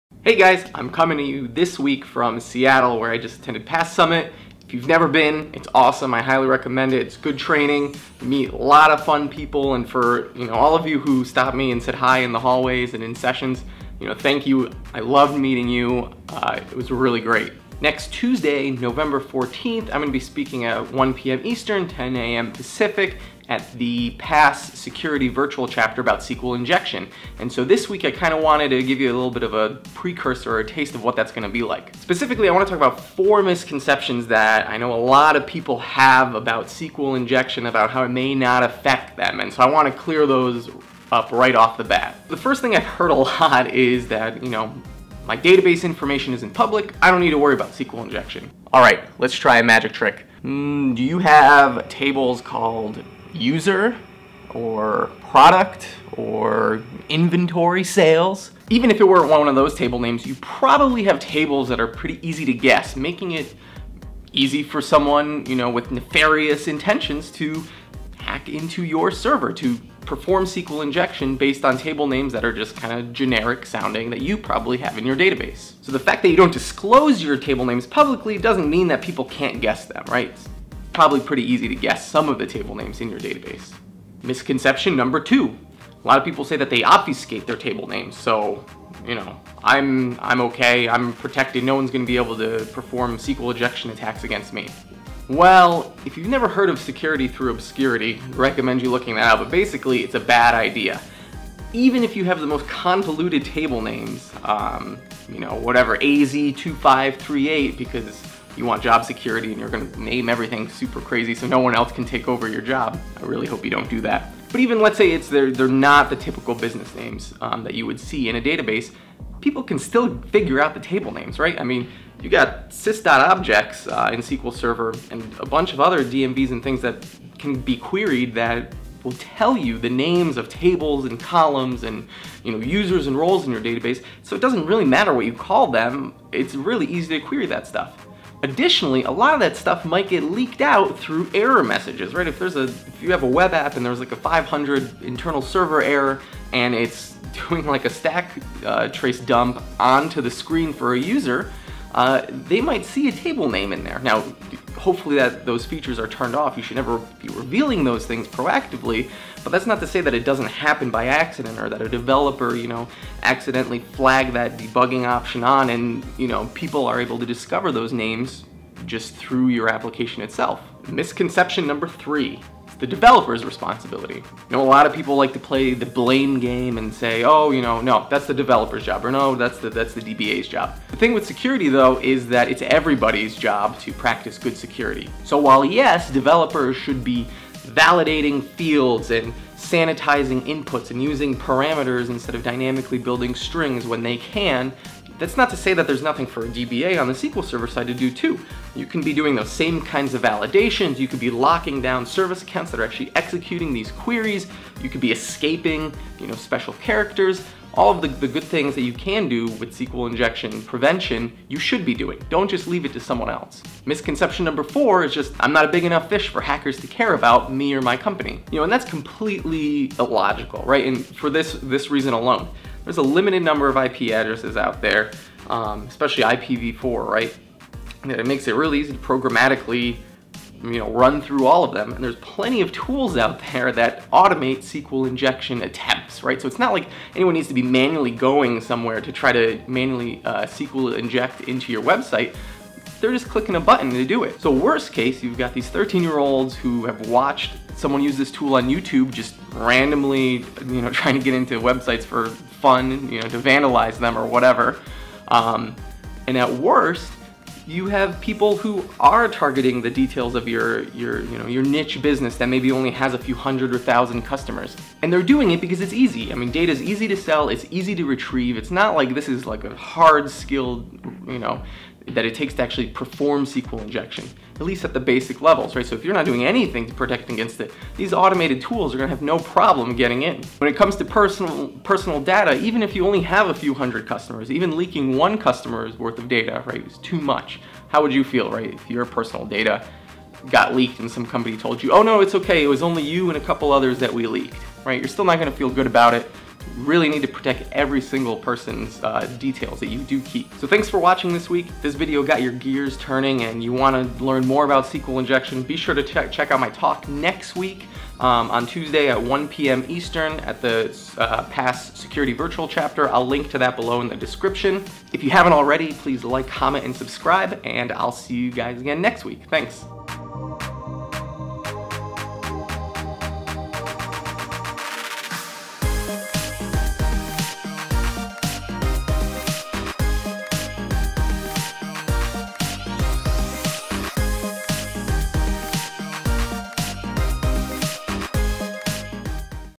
4 Misconceptions About SQL Injection
SQL injection continues to be one of the leading reasons for data breaches.

Many people (incorrectly) assume that after more than 20 years of exploits in the wild, automatic protections MUST exist against SQL injection.

In this week's video we take a look at why that and some other beliefs about SQL injection are incorrect.

Presenting November 14, 2017 at 1PM Eastern about preventing SQL injection in your databases.

Want to receive my latest weekly blog posts and videos in your inbox?